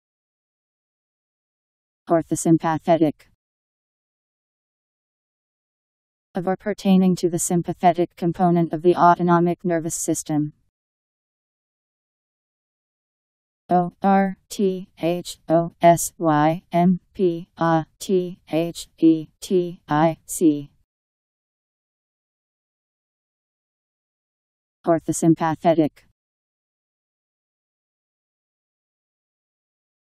Orthosympathetic Meaning
Video shows what orthosympathetic means. Of or pertaining to the sympathetic component of the autonomic nervous system.  Orthosympathetic Meaning. How to pronounce, definition audio dictionary. How to say orthosympathetic. Powered by MaryTTS, Wiktionary